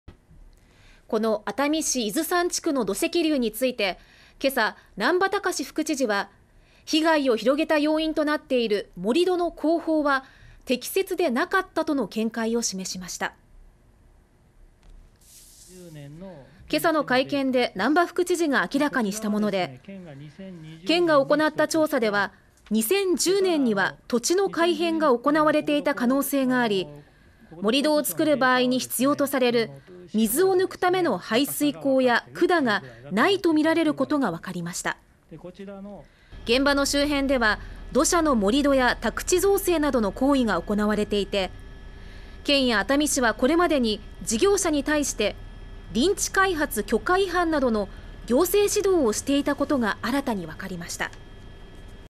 「盛り土の工法は適切でなかった…」 静岡・熱海市の土石流災害 難波副知事が会見
静岡県熱海市伊豆山地区の土石流について7日朝、難波喬司副知事は被害を広げた要因となっている盛り土の工法は適切でなかったとの見解を示しました。

　けさの会見で難波副知事があきらかにしたもので、県が行った調査では2010年には土地の改変が行われていた可能性があり、盛り土を作る場合に必要とされる水を抜くための排水溝や、管がないとみられることがわかりました。
　現場の周辺では土砂の盛り土や宅地造成などの行為が行われていて、県や熱海市はこれまでに事業者に対して、臨時開発許可違反などの是正勧告をしていたことが新たに分かりました。